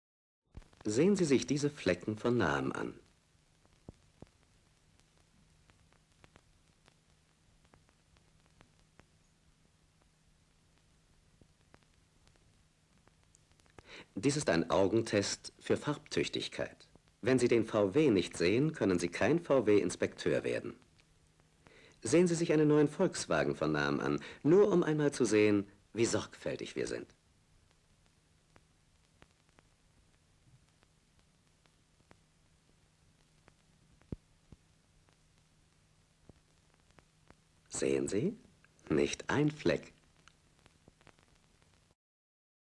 Classic early 70's vw beetle commercial
From the DVD 'Legend on wheels'. Wanna have this DVD packed with 2 hours of high quality digitally remastered old vw commercials? The DVD really brings you back to the '60s and '70s.